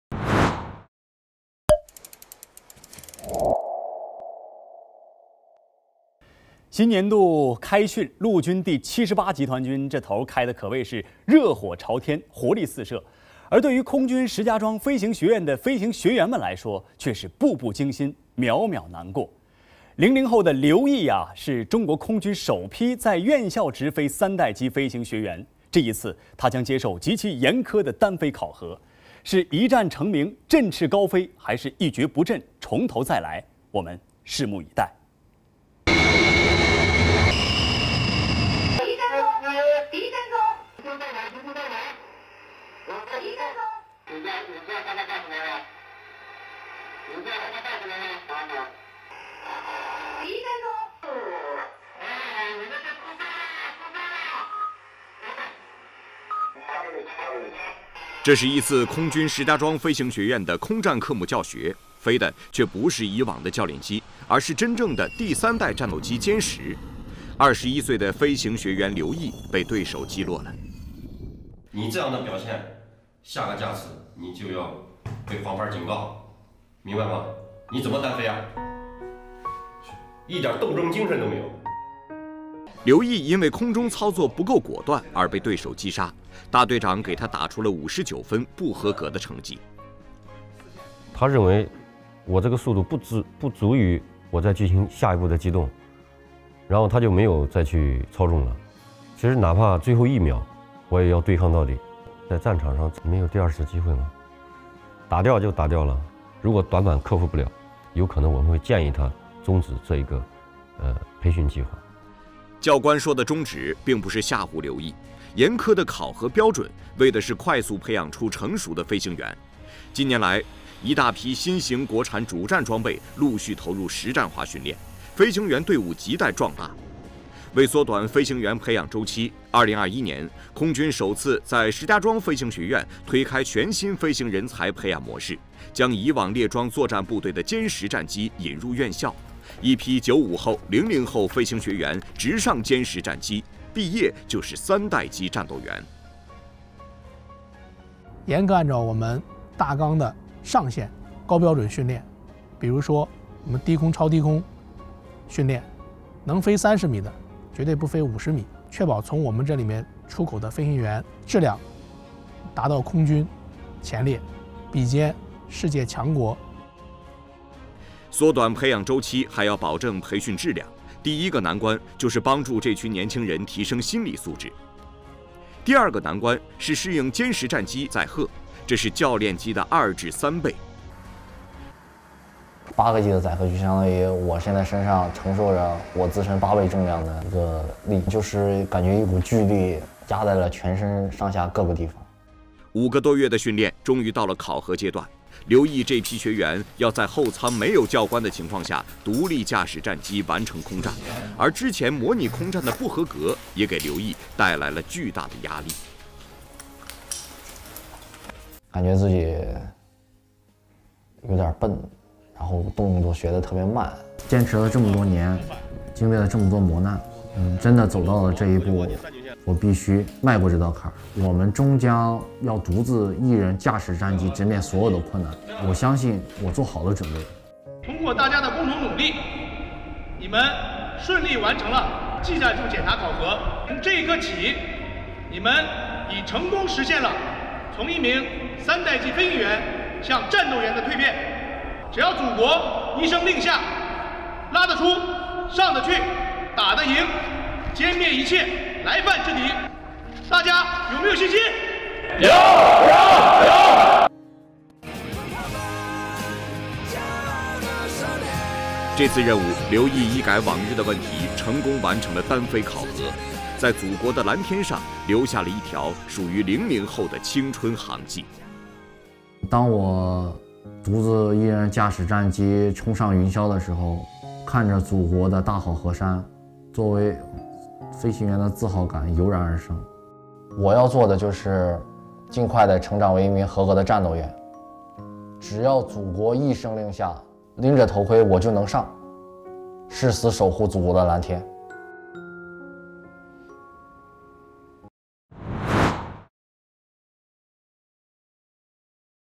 【新年新气象·新力量开跋】空军首批院校培养直飞三代机飞行员完成单飞考核
新年度开训对空军石家庄飞行学院的飞行学员们来说却是步步惊心，秒秒难过。零零后的刘艺是中国空军首批在院校直飞三代机飞行学员，这一次他将接受极其严苛的单飞考核，是"一战成名真翅高飞"还是"一蹶不振重头再来"，我们拭目以待。